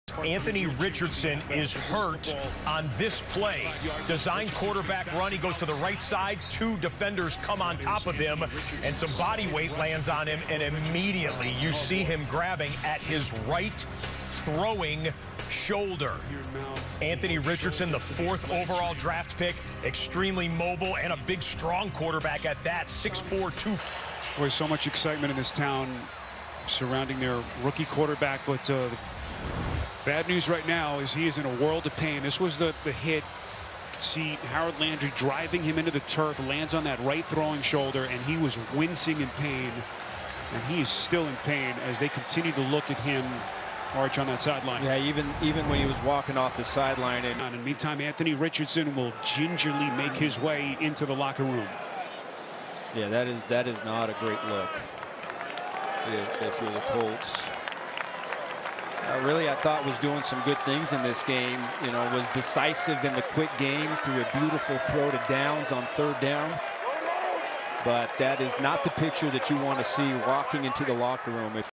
Anthony Richardson suffers shoulder injury vs. Titans
prayers